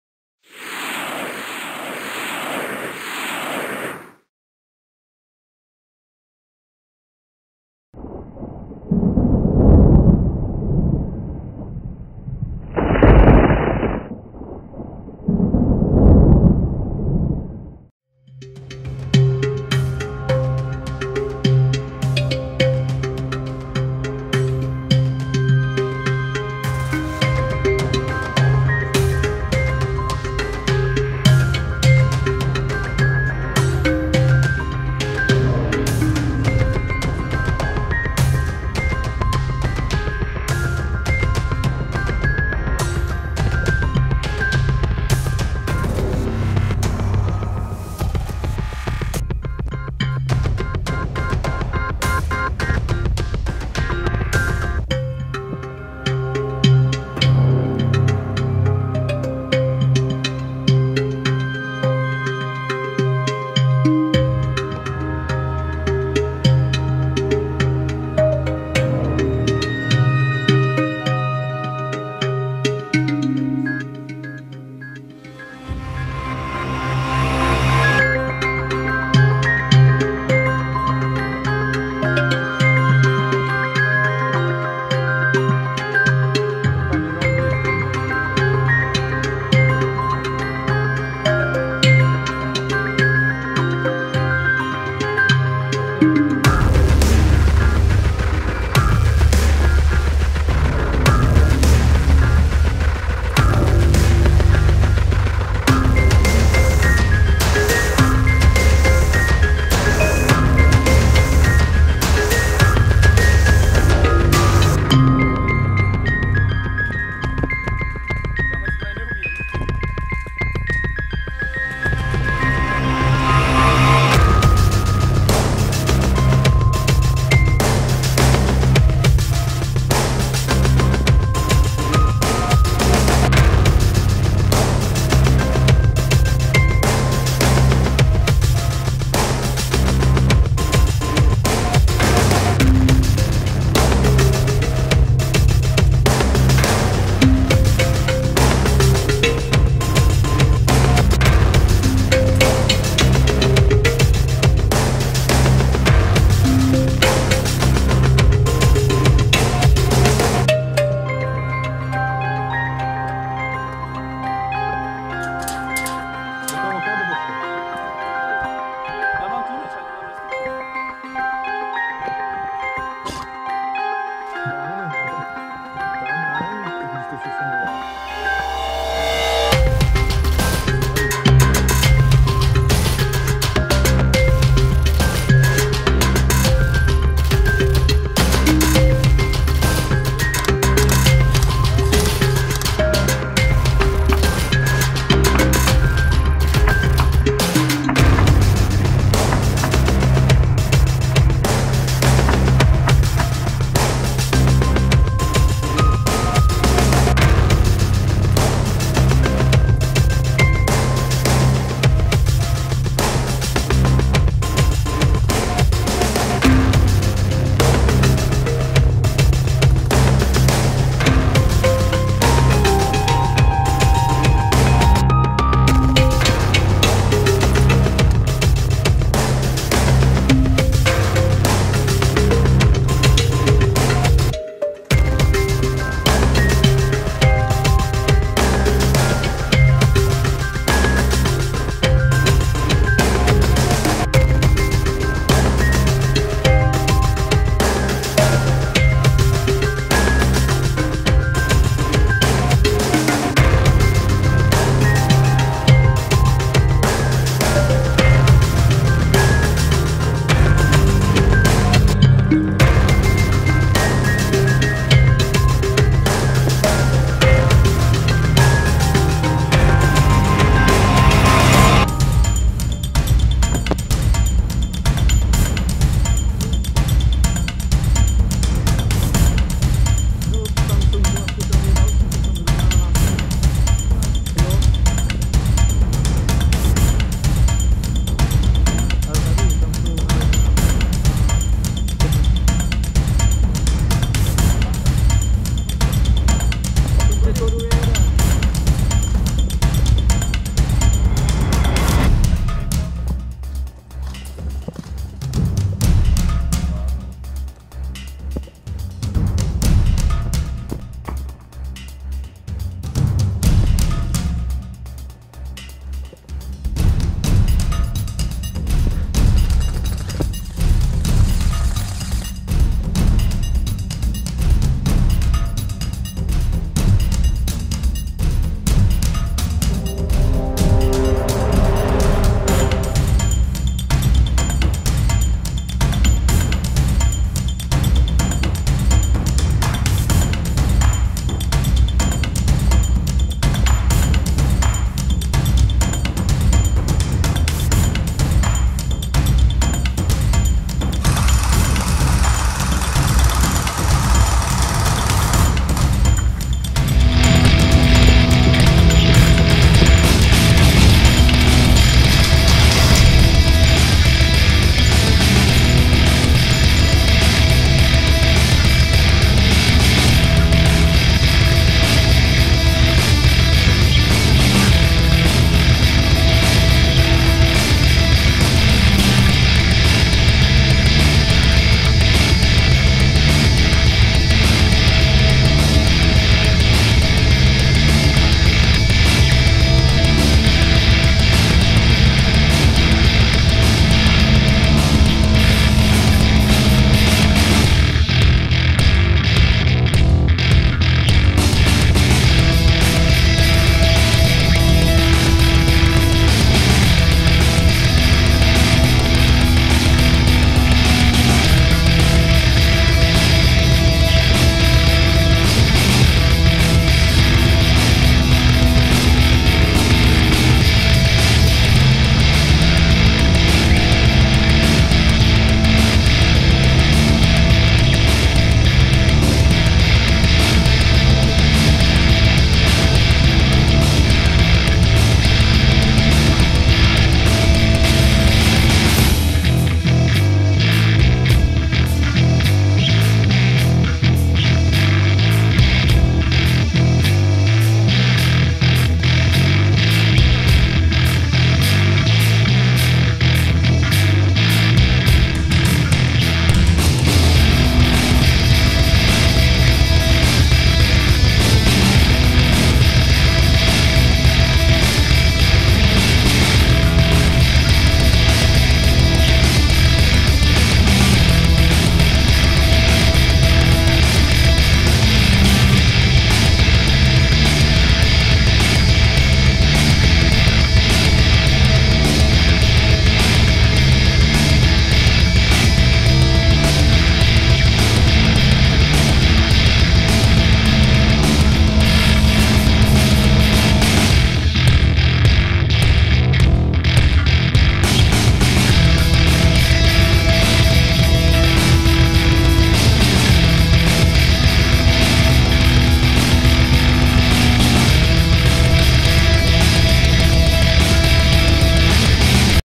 Mogadisho VIP
Video z airsoftové akce Mogadisho VIP natočené a sestříhané týmem 4.SPB.